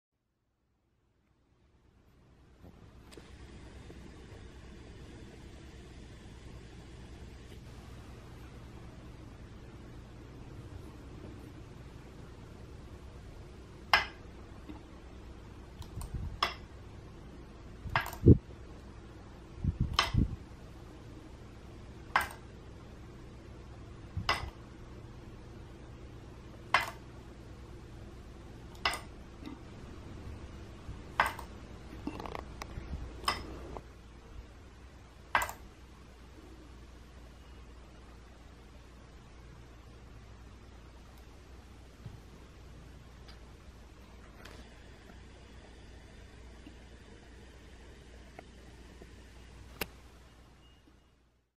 Simple Automata #7 SeeSaw Birds
Simple Automata Debating Birds inspired by Robert Race's Talking Birds.